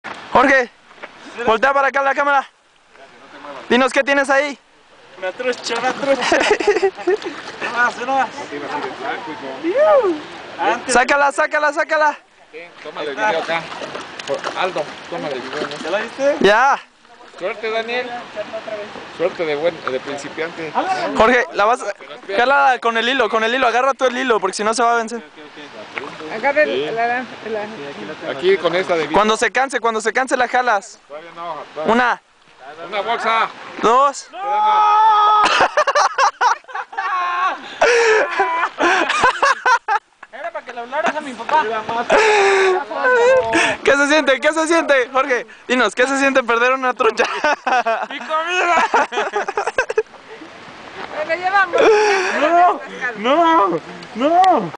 Trucha Fugitiva
Cuando fuimos a los prismas basálticos y luego a pescar..fue toda una hazaña! y yo dejé ir "voluntariamente" mi comida..sniff..jaja